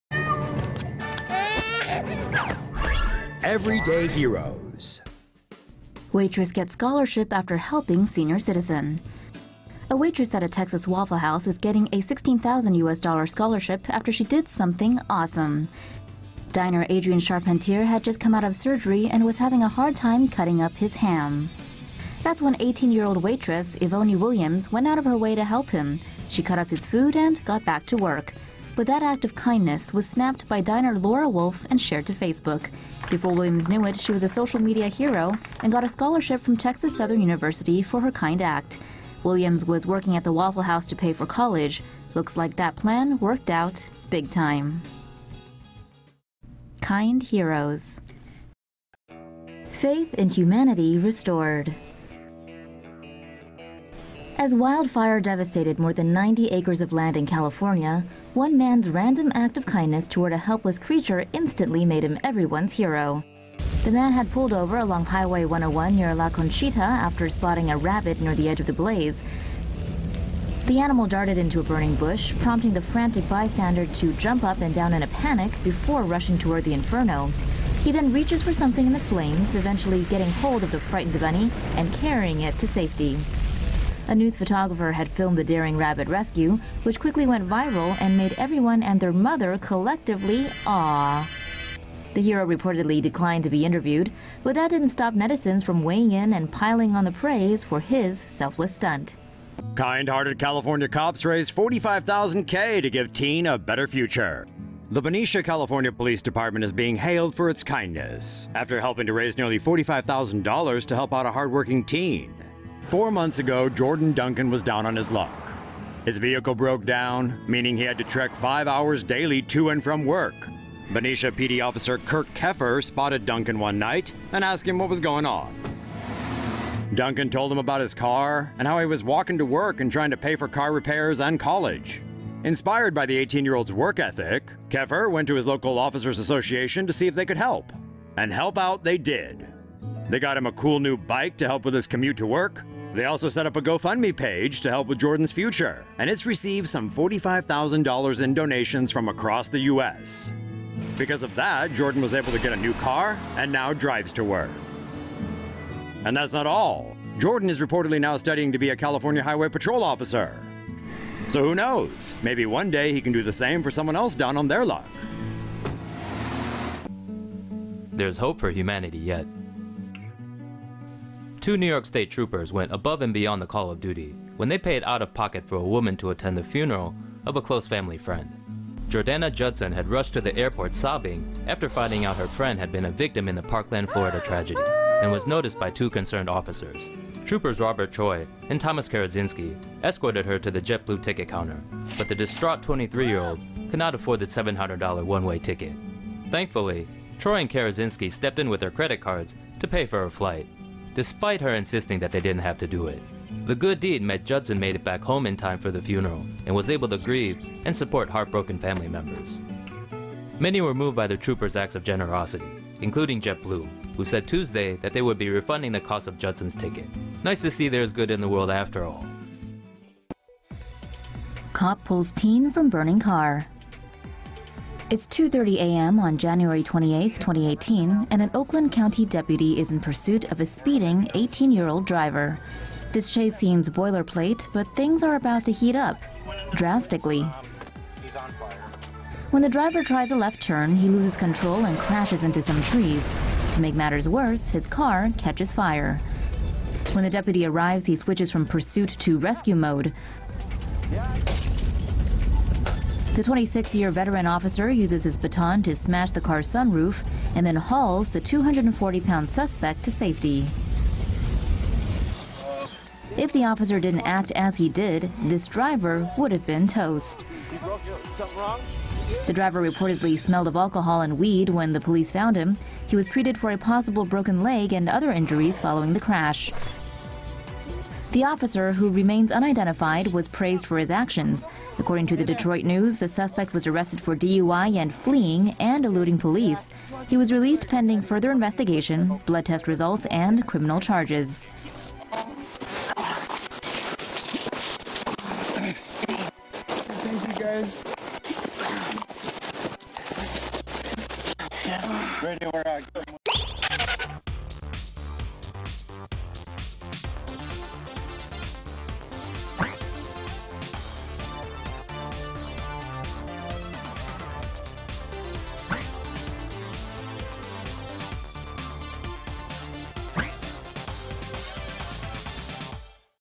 Acts of kindness rewarded: Humane waitress helps old man cut his food, gets scholarship - TomoNews
LA MARQUE, TEXAS — A waitress at a Texas waffle house is getting a $16,000 scholarship after she did something awesome.